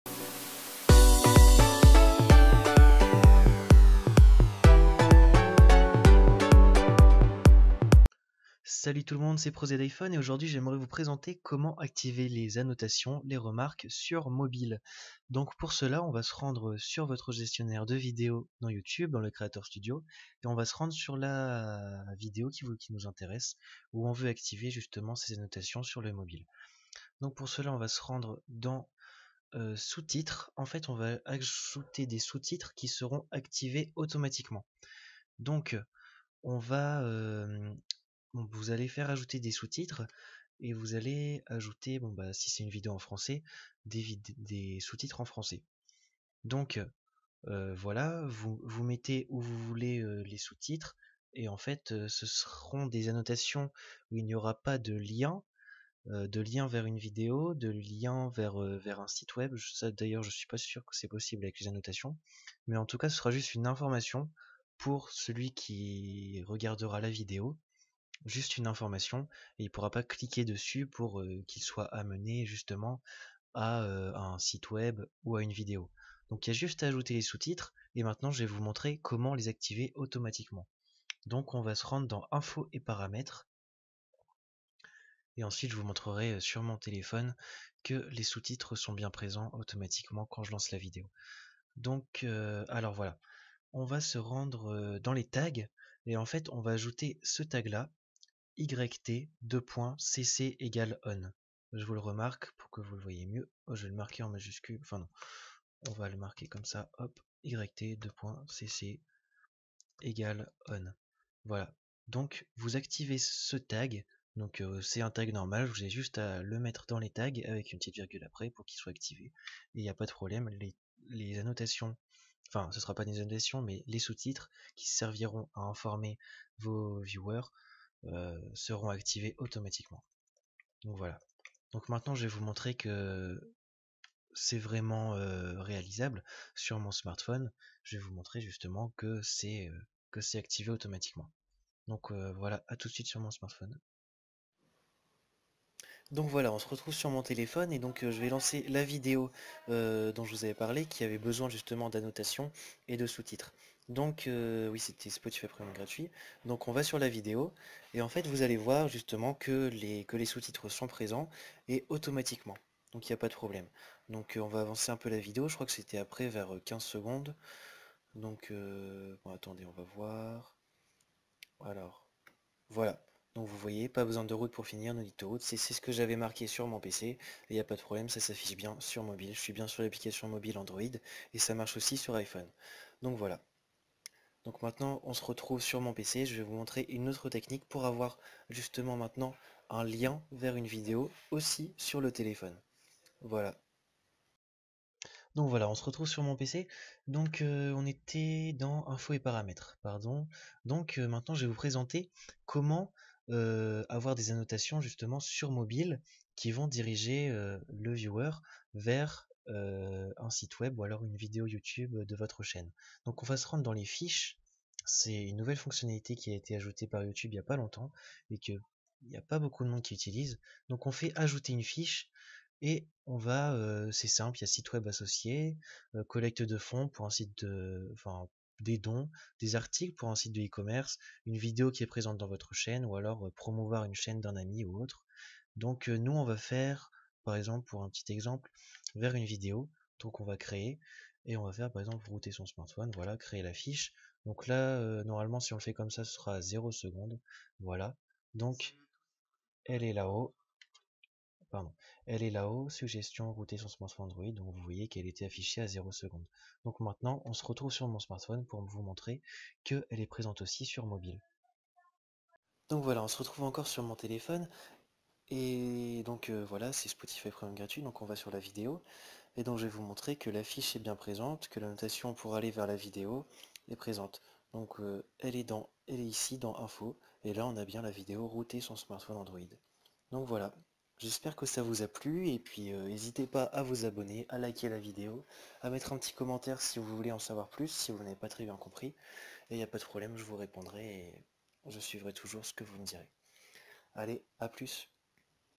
[Android/iOS] Annotations YouTube Mobile
Hello tout le monde, aujourd'hui je vous présente comment intégrer des annotations sur les téléphones mobiles, que ce soit sur Android ou iOS grâce aux sous-titres et aux fiches.